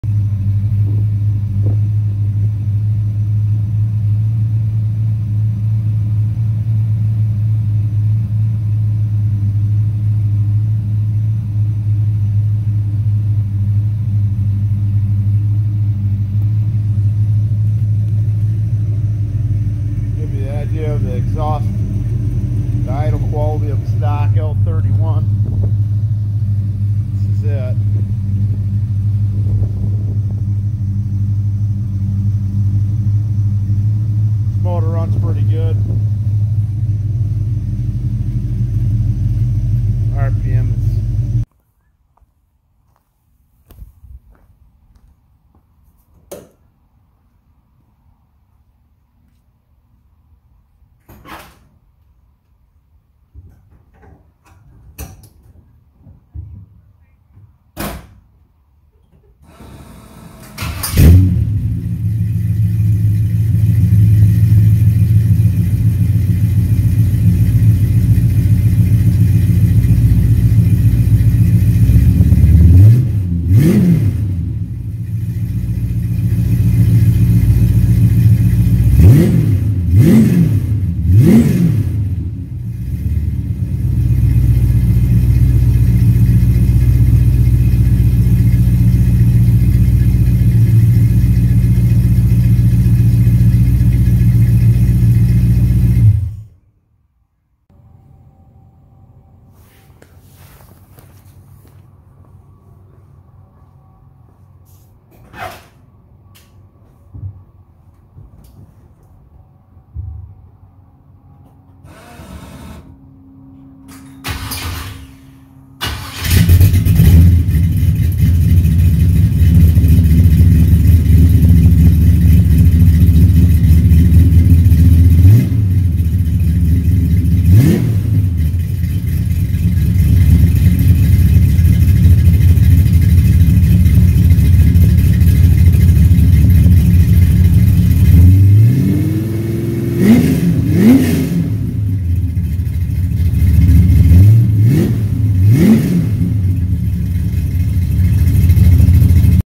Idle 350 Chevy Vortec Heads comp cams 292 vs GM Performance Crate 350 HO 330 HP vs Stock 350 vortec
Idle comparrisons of a stock L31 vortec 350, crate vortec 350ho and vortec with a comp cams 292h